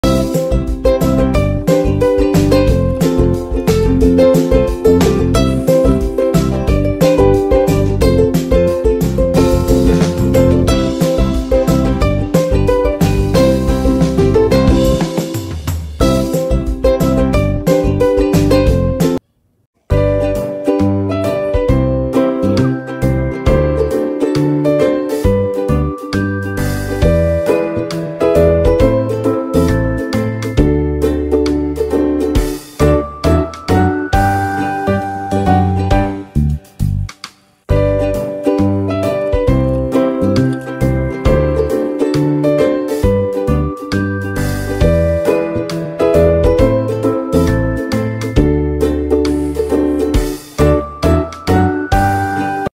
Hermit crab shell guide
A guide for mainly purple pincher shell choosing comment what video I should make next